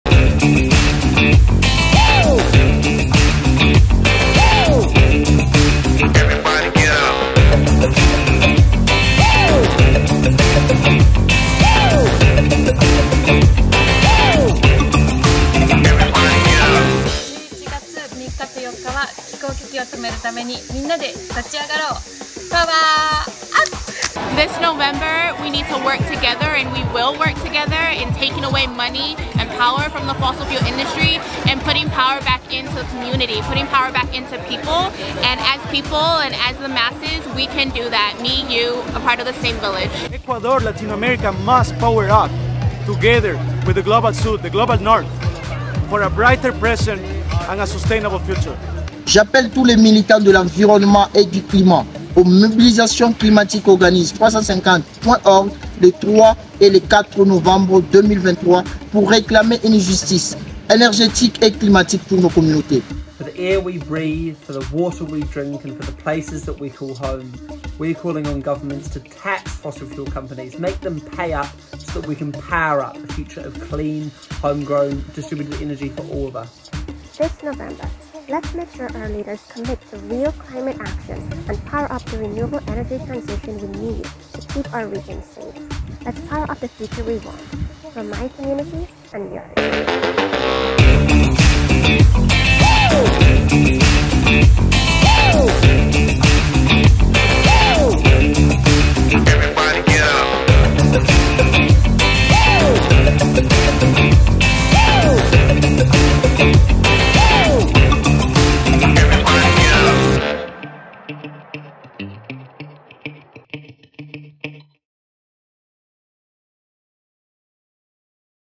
Ayo, Gabung Global Power Up Indonesia!
Kepada kamu yang peduli akan masa depan tanpa krisis iklim.

Kita akan menuntut kandidat Presiden 2024 untuk memiliki komitmen ambisius dalam penanganan perubahan iklim dan transisi energi yang berkeadilan!

Di berbagai penjuru dunia, orang-orang akan berkumpul untuk mendorong revolusi energi terbarukan global. Ayo alihkan dana dan pengaruh politik dari perusahaan-perusahaan energi kotor menuju masa depan yang bersih dan lebih adil untuk semua!

Gabung dalam gerakan Global Power Up!